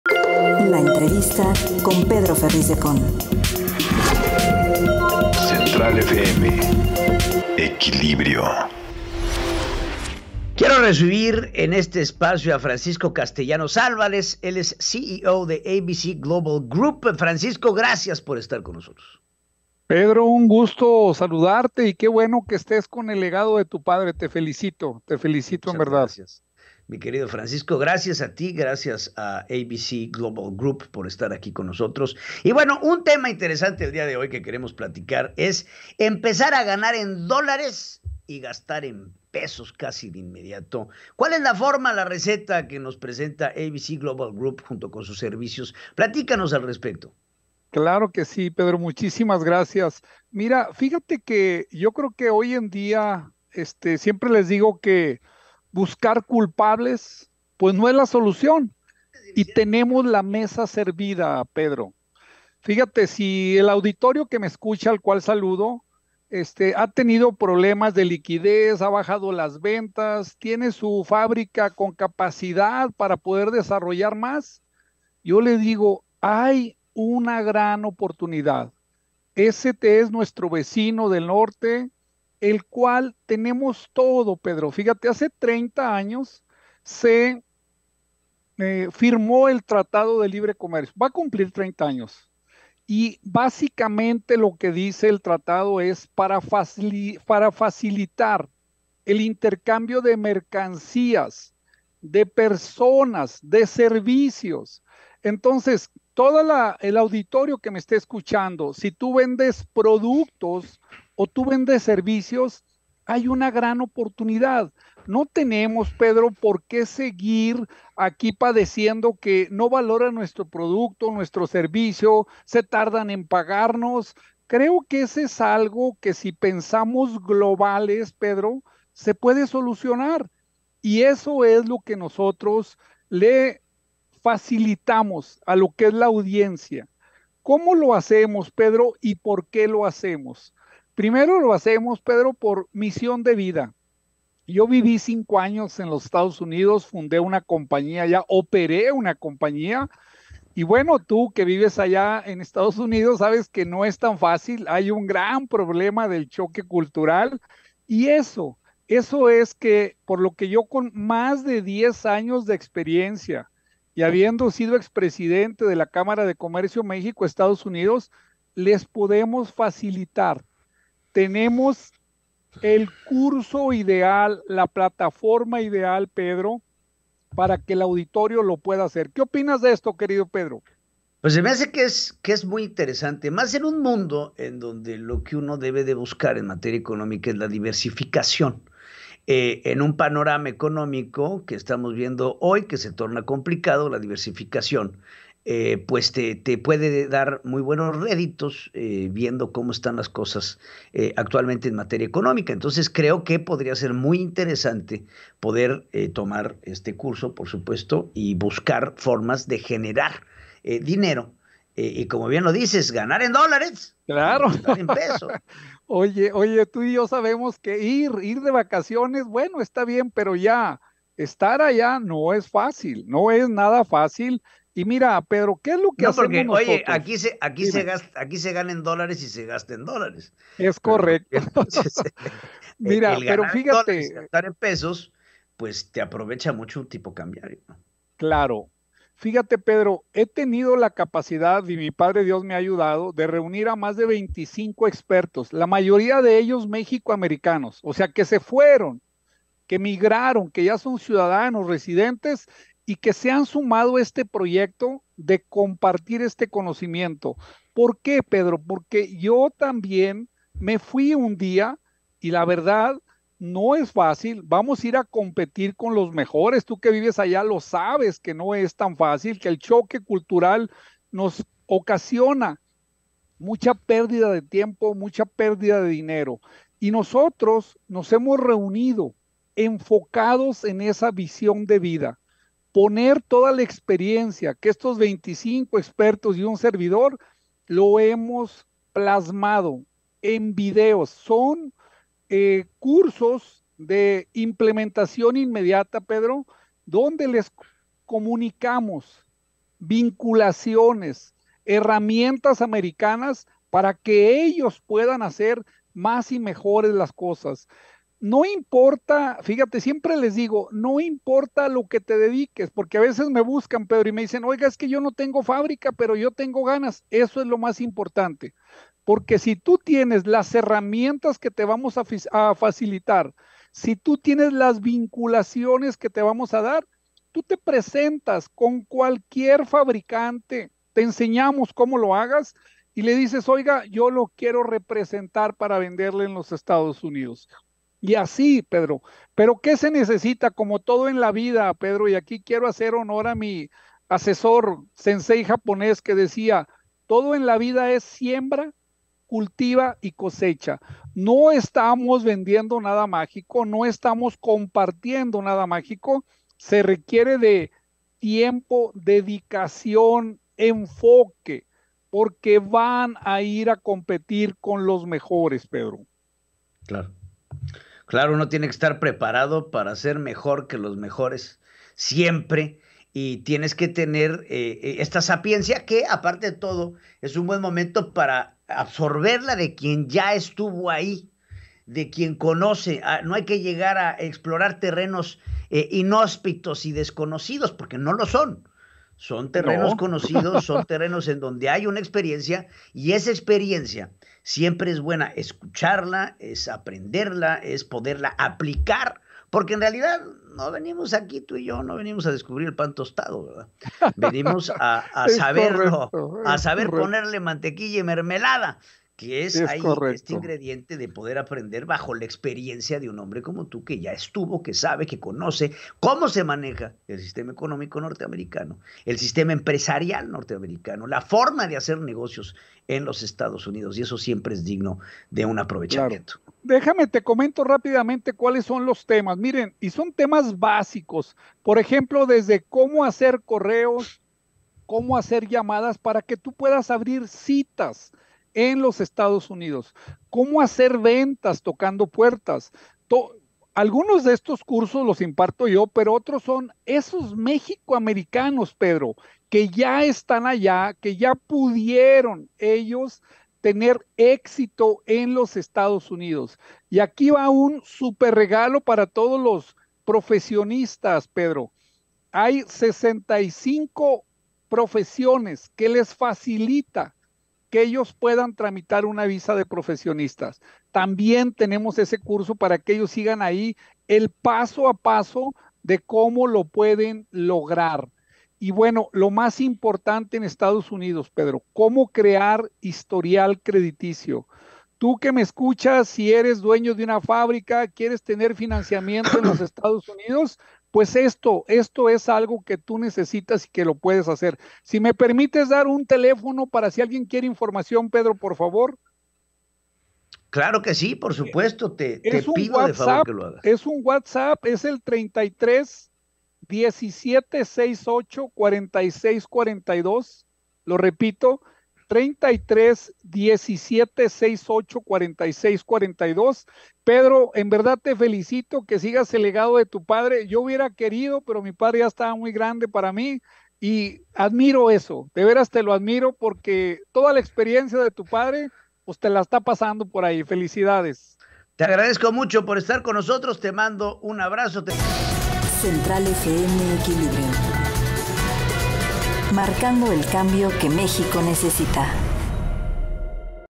¿Cómo empezar a ganar en dólares y gastar en pesos?
En entrevista con Pedro Ferriz Híjar para Central Equilibrio, nuestro experto en finanzas, Francisco Castellanos, CEO de ABC Global Group, nos habla sobre cómo podemos empezar a ganar en dólares y gastar en pesos casi de forma inmediata.

El aprendizaje va de la mano de 25 expertos mexicoamericanos, que nos enseñan a explotar nuestras áreas de oportunidad.

CONTACTO Central Equilibrio:

Conoce sobre nuestro proyecto por la unidad de México